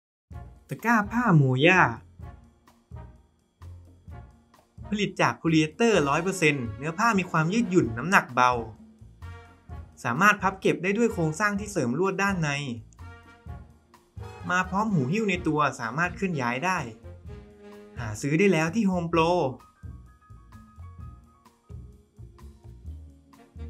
ตะกร้าผ้า MOYA (SKU : 1234165) 🎉
จัดเก็บของใช้ภายในบ้านได้อย่างเป็นระเบียบมากยิ่งขึ้นด้วยตะกร้าดีไซน์น่ารัก จาก MOYA วัสดุผลิตจากโพลีเอสเตอร์ที่มีน้ำหนักเบา เคลื่อนย้ายสะดวก และแข็งแรงทนทานต่อการใช้งานด้วยโครงสร้างที่เสริมลวด ช่วยให้ตะกร้าไม่เสียรูปทรง สามารถพับเก็บได้ เหมาะสำหรับใช้เป็นตะกร้าเก็บเสื้อผ้า, ของใช้ขนาดเล็ก หรือสิ่งของที่มีน้ำหนักเบา 💕

✨ ผลิตจากโพลีเอสเตอร์ 100% เนื้อผ้ามีความยืดหยุ่น น้ำหนักเบา และมีอายุการใช้งานยาวนาน
✨ ตะกร้ามีความแข็งแรง อยู่ทรง และสามารถพับเก็บได้ด้วยโครงสร้างที่เสริมลวดด้านใน
✨ มาพร้อมหูหิ้วในตัว สามารถเคลื่อนย้าย หรือยกผ้าไปซักได้อย่างสะดวก
✨ ช่วยจัดเก็บเสื้อที่ใส่แล้วให้เป็นระเบียบเรียบร้อย
✨ มีลวดลายสวยงาม และสีสันสดใส ตั้งวางใช้งานได้อย่างลงตัวทุกมุมห้อง

ช่องทางสำหรับติดตามโฮมโปร